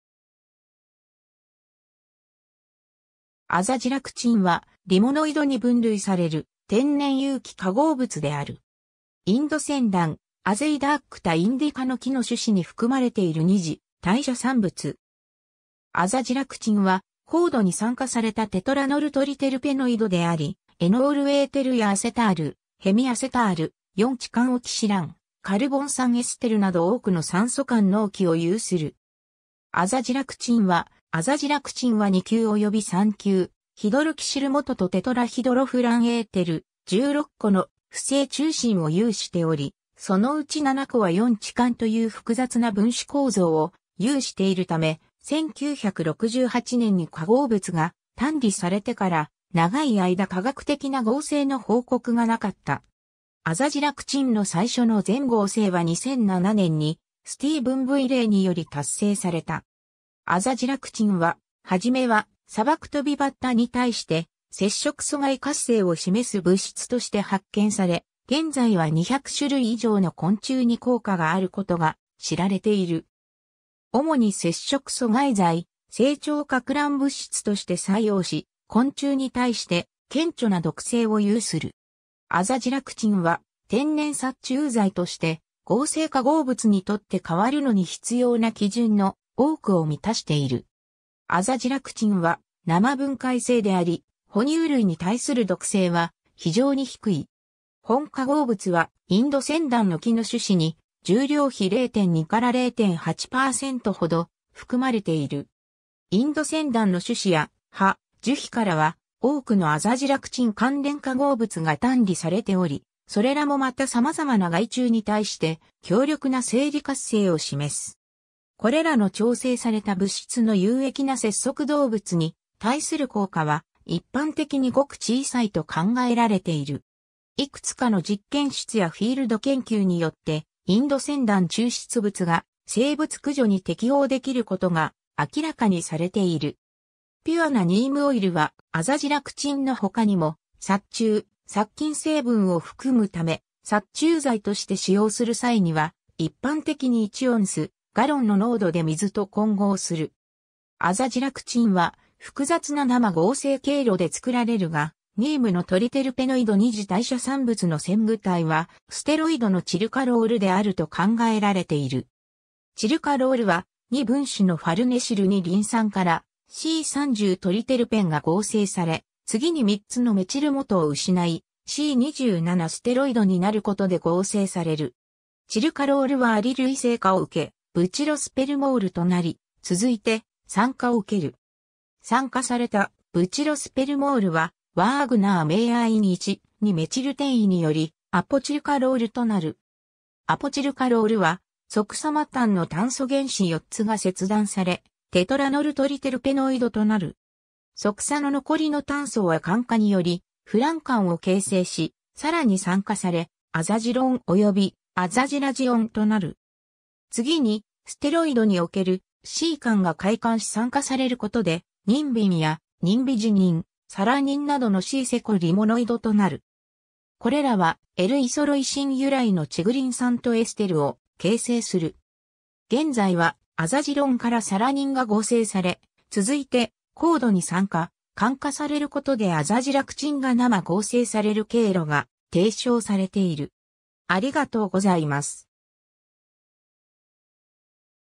アザジラクチン
アザジラクチン（azadirachtin）はリモノイドに分類される天然有機化合物である。
インドセンダン Azadirachta indica（ニーム）の木の種子に含まれている二次代謝産物。
アザジラクチンは高度に酸化されたテトラノルトリテルペノイドであり、エノールエーテルやアセタール、ヘミアセタール、四置換オキシラン、カルボン酸エステルなど多くの酸素官能基を有する。
 アザジラクチンはアザジラクチンは二級および三級ヒドロキシル基とテトラヒドロフランエーテル、16個の不斉中心を有しており、その内7個は4置換という複雑な分子構造を有しているため、1968年に化合物が単離されてから、長い間化学的な合成の報告がなかった。
 アザジラクチンの最初の全合成は2007年にスティーヴン・V・レイ（ケンブリッジ大学）により達成された。
 アザジラクチンは、初めはサバクトビバッタ (Schistocerca gregaria) に対して摂食阻害活性を示す物質として発見され、現在は200種類以上の昆虫に効果があることが知られている。
主に摂食阻害剤、成長撹乱物質として作用し、昆虫に対して顕著な毒性を有する（LD50 (S. littoralis): 15 μg/g）。
アザジラクチンは天然殺虫剤として合成化合物に取って換わるのに必要な基準の多くを満たしている。
アザジラクチンは生分解性であり（光と水に曝らされると100時間以内に分解する）、ほ乳類に対する毒性は非常に低い（マウスに対するLD50は」 3,540 mg/kgであり実質的に無毒）。
 本化合物はインドセンダン (Azadirachta indica、ニーム) の木の種子に重量比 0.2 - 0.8 %程含まれている（したがって、化合物名のAzaは、炭素原子が窒素原子に置換されたことを示す接頭辞ではなく、学名に由来する）。
インドセンダンの種子や葉、樹皮からは多くのアザジラクチン関連化合物が単離されており、それらもまた様々な害虫に対して強力な生理活性を示す。
 これらの調整された物質の有益な節足動物に対する効果は一般的にごく小さいと考えられている。
いくつかの実験室やフィールド研究によって、インドセンダン抽出物が生物駆除に適応できることが明らかにされている。
ピュアなニームオイルはアザジラクチンの他にも殺虫、殺菌成分を含むため、殺虫剤として使用する際には一般的に 1オンス/ガロン（7.6 mL/L）の濃度で水と混合する。
 アザジラクチンは複雑な生合成経路で作られるが、ニームのトリテルペノイド二次代謝産物の前駆体はステロイドのチルカロール (tirucallol) であると考えられている。
チルカロールは、2分子のファルネシル二リン酸 (FPP) からC30トリテルペンが合成され、次に3つのメチル基を失いC27ステロイドになることで合成される。
チルカロールはアリル異性化を受けブチロスペルモール (butyrospermol) となり、続いて酸化を受ける。
酸化されたブチロスペルモールはワーグナー・メーヤワイン1,2-メチル転位によりアポチルカロール (apotirucallol) となる。
 アポチルカロールは側鎖末端の炭素原子4つが切断されテトラノルトリテルペノイドとなる。
側鎖の残りの炭素は環化によりフラン環を形成し、さらに酸化されアザジロン (azadirone) およびアザジラジオン (azadiradione) となる。
次にステロイドにおけるC環が開環し酸化されることでニンビン (nimbin)や、ニンビジニン (nimbidinin)、サランニン (salannin) などのC-seco-リモノイドとなる。
これらはL--イソロイシン由来のチグリン酸とエステルを形成する。
現在は、アザジロンからサラニンが合成され、続いて高度に酸化、環化されることでアザジラクチンが生合成される経路が提唱されている。